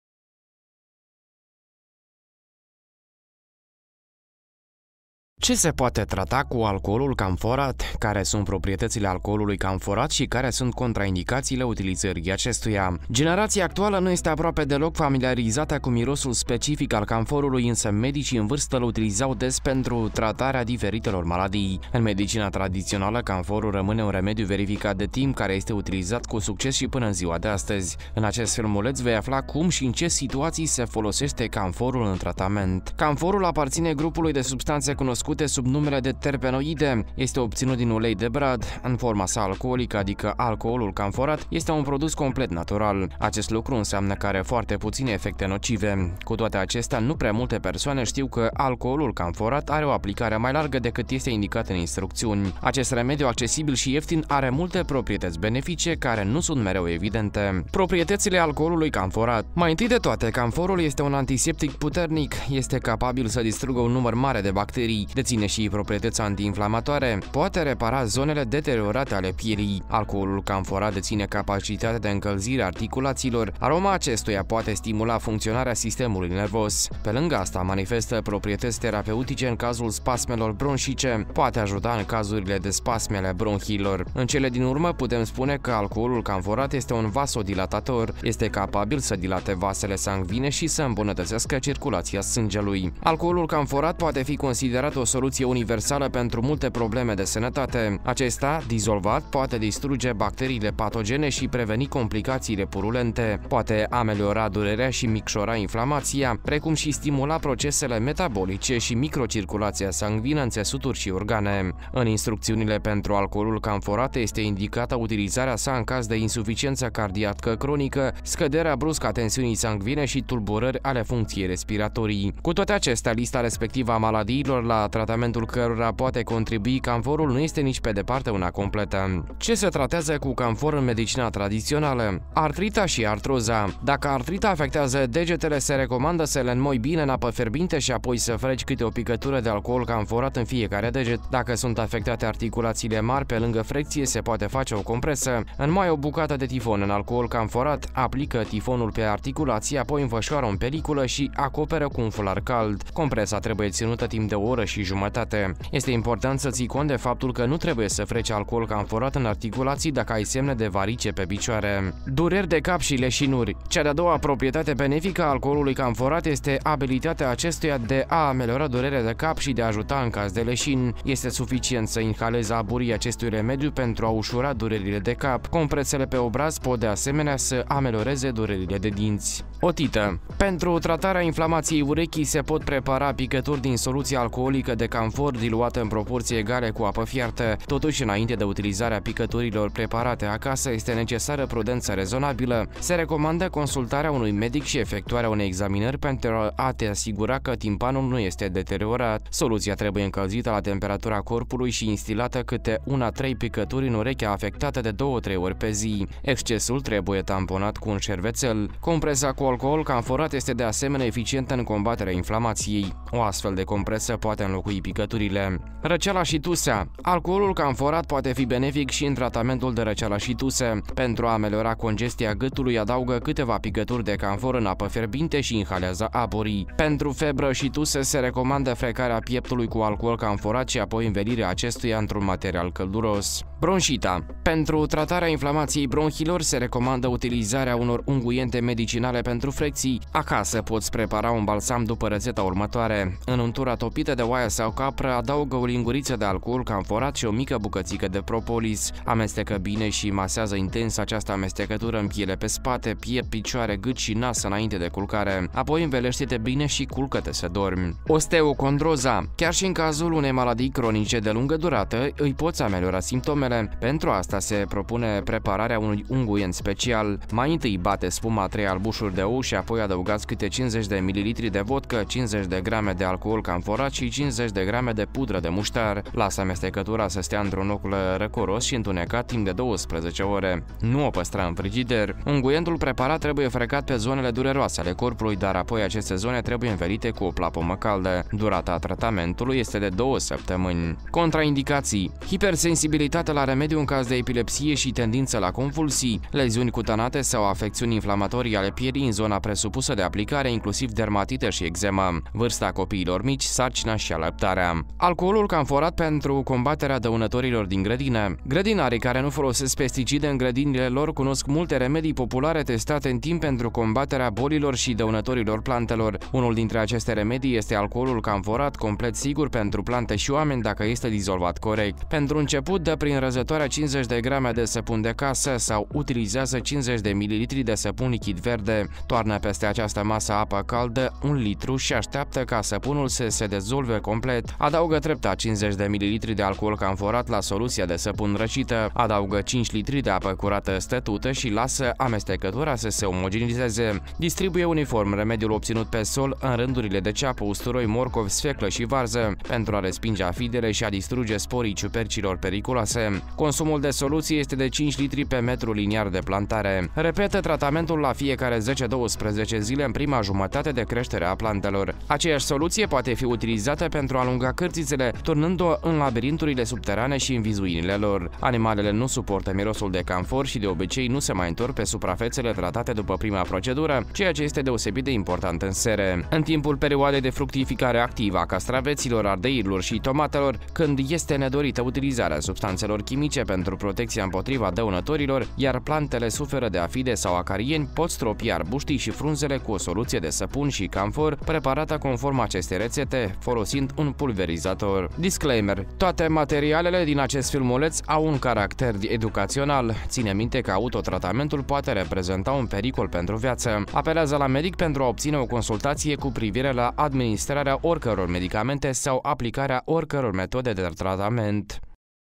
EFECTELE UIMITOARE ALE ALCOOLULUI CAMFORAT
Ce se poate trata cu alcoolul camforat? Care sunt proprietățile alcoolului camforat și care sunt contraindicațiile utilizării acestuia?
Generația actuală nu este aproape deloc familiarizată cu mirosul specific al camforului, însă medicii în vârstă îl utilizau des pentru tratarea diferitelor maladii. În medicina tradițională, camforul rămâne un remediu verificat de timp, care este utilizat cu succes și până în ziua de astăzi. În acest filmuleț, vei afla cum și în ce situații se folosește camforul în tratament.
Camforul aparține grupului de substanțe cunoscute sub numele de „terpenoide”; este obținut din ulei de brad. În forma sa alcoolică, adică alcoolul camforat, este un produs complet natural. Acest lucru înseamnă că are foarte puține efecte nocive. Cu toate acestea, nu prea multe persoane știu că alcoolul camforat are o aplicare mai largă, decât este indicat în instrucțiuni. Acest remediu accesibil și ieftin are multe proprietăți benefice, care nu  sunt mereu evidente.